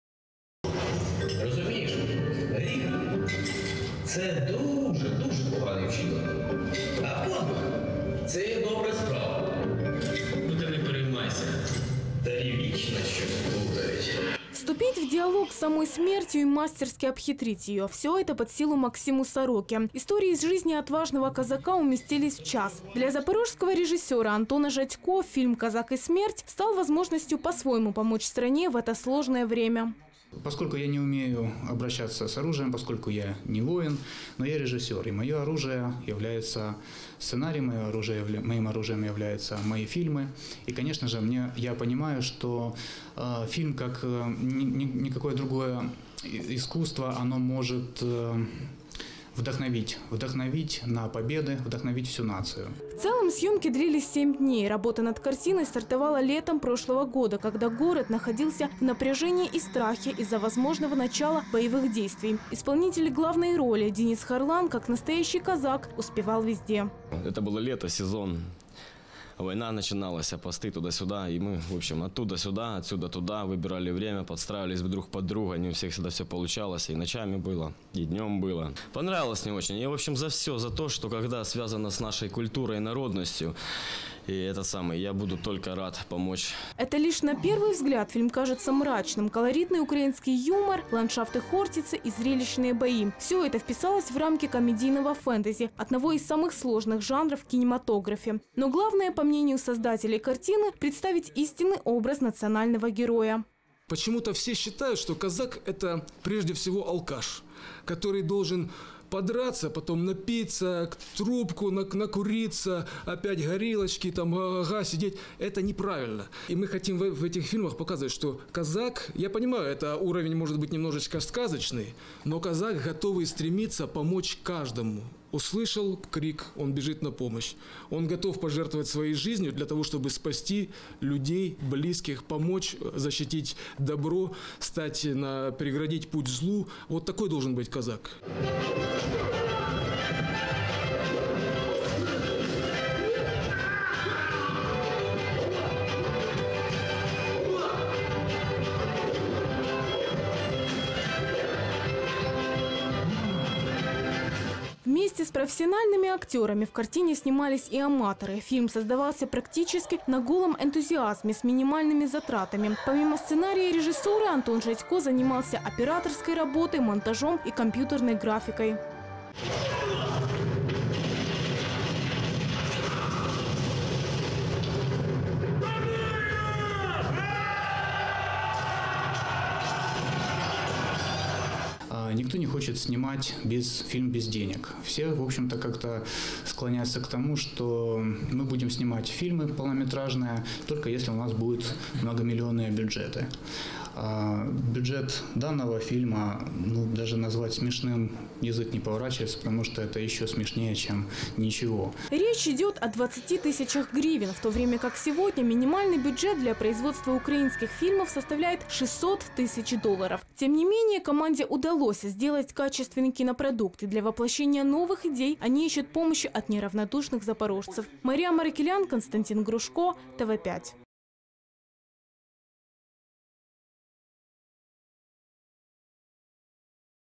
Козак та смерть
В кинотеатре им. Довженко состоялся премьерный показ картины «Козак и смерть». Это полнометражный фильм запорожского режиссера Антона Жадько. Несмотря на дорогой и сложный жанр фэнтези, режиссеру удалось создать зрелищное кино. деньги на съемку фильма были собраны неравнодушными к отечественному кинематографу людьми.